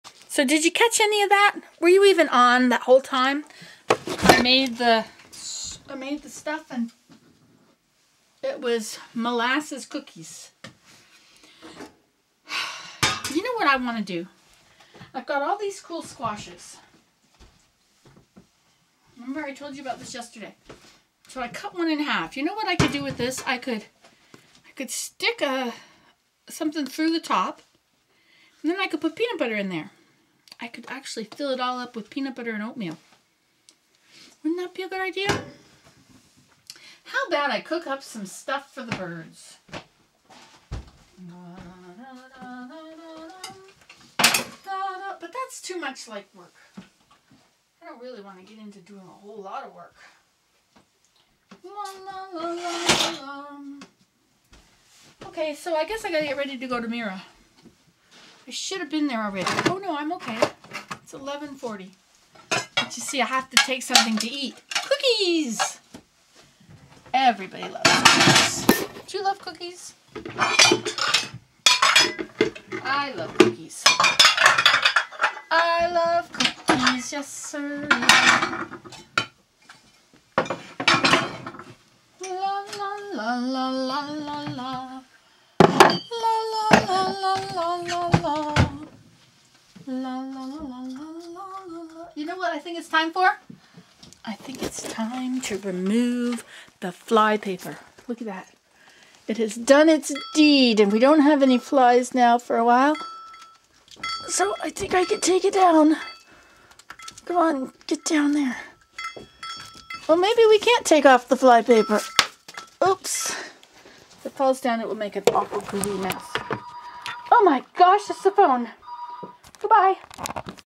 Nancy Today: Time to take down the flypaper ASMR
Just putting away dishes, talking, doing this and that while the cookies bake.